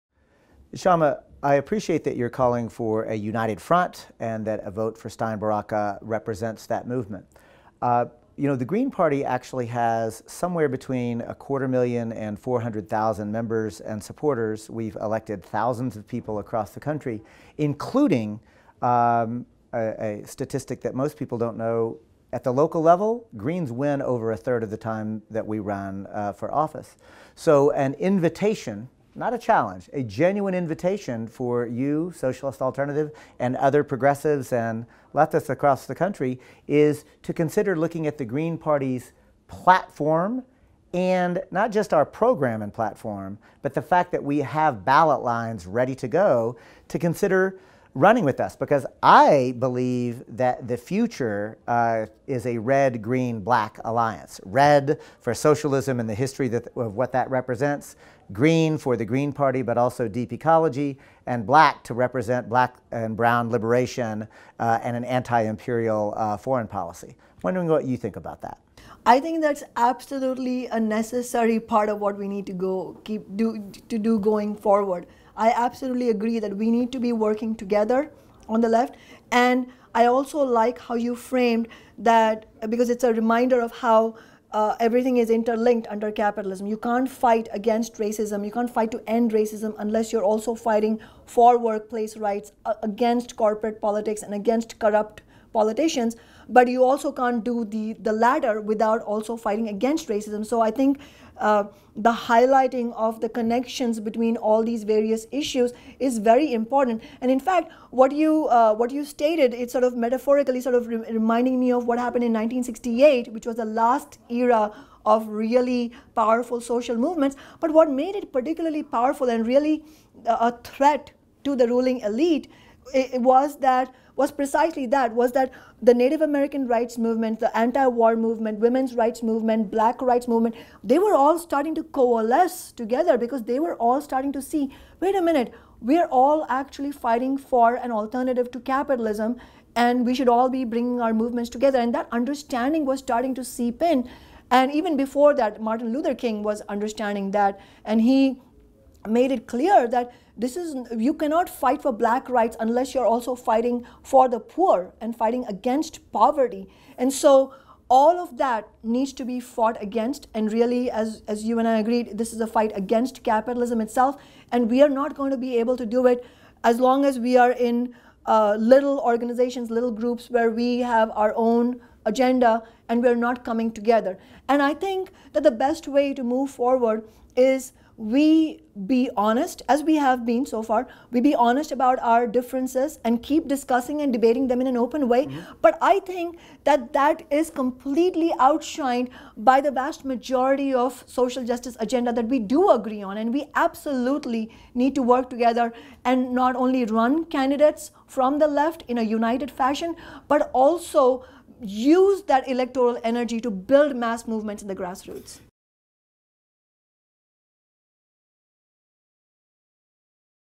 Conversation with Kshama Sawant, Part 1
Stein/Baraka campaign manager David Cobb sat down with Socialist Alternative spokesperson Kshama Sawant to discuss her desire to see various left-wing group coalesce around the Green Party's 2016 presidential ticket.